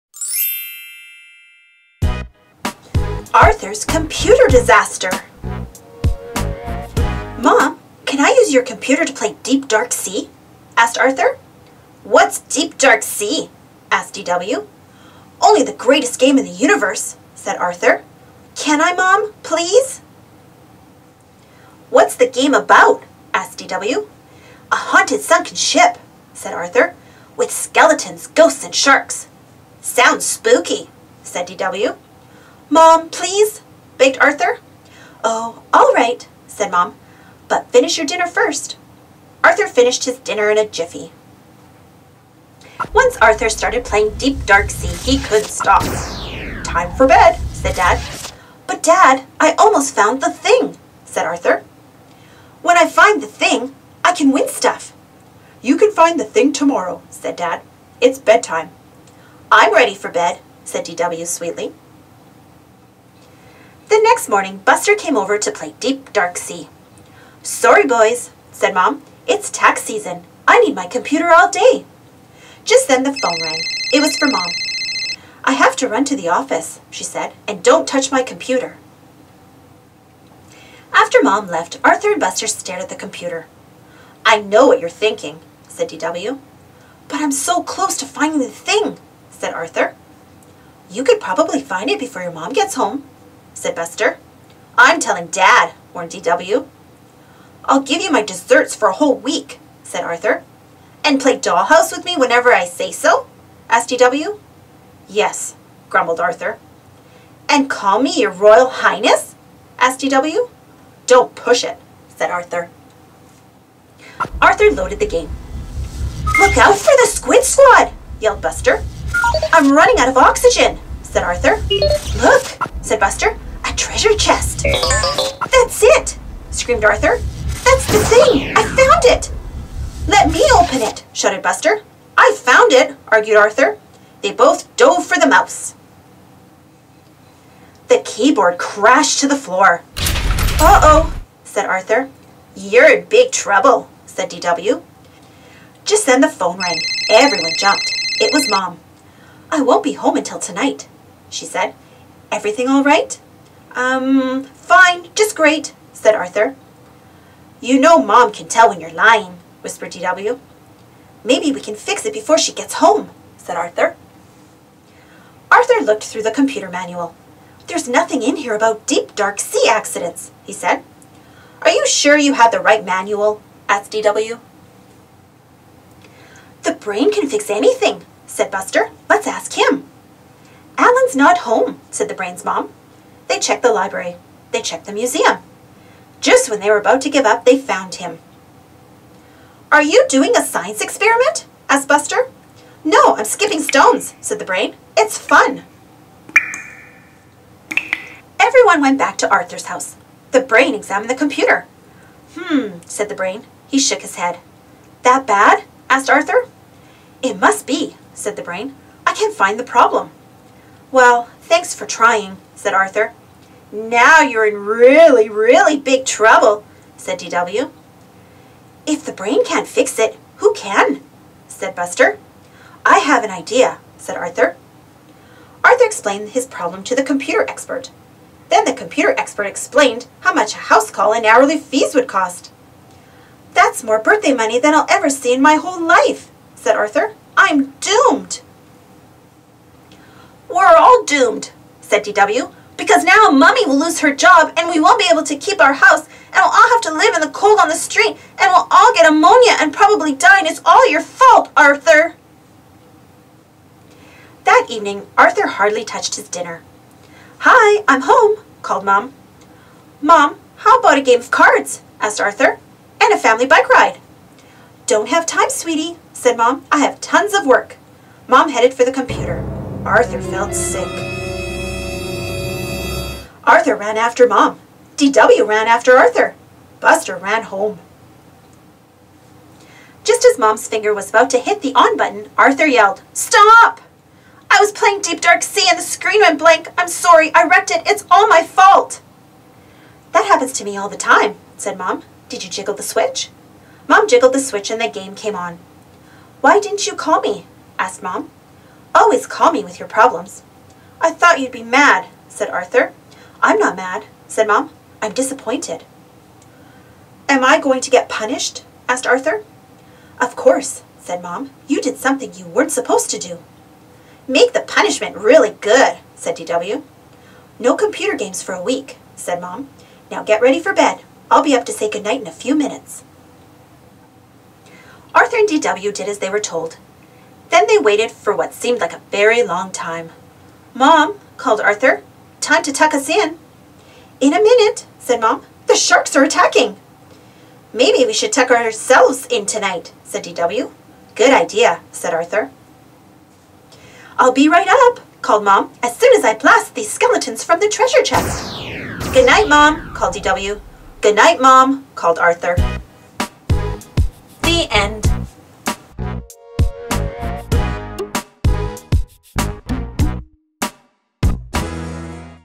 ARTHUR'S COMPUTER DISASTER
Arthur was not aloud to play on his mom's computer but he did anyway. The computer stopped working! Can Buster and Arthur fix it before his mom get's home? For all ages.

By: Marc Brown
Copyright Marc Brown